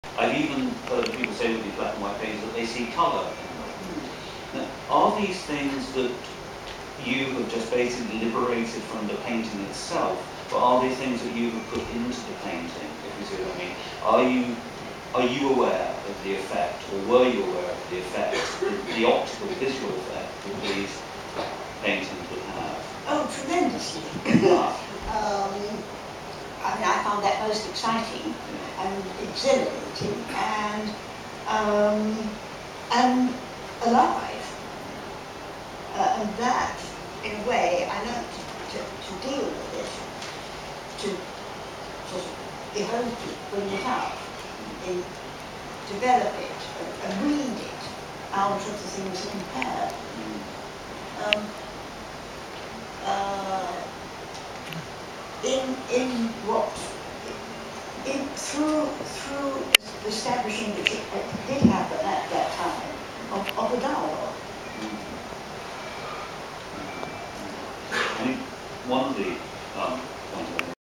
Bridget Riley 2
Bridget Riley in conversation with Michael Bracewell
Frieze Art Fair
London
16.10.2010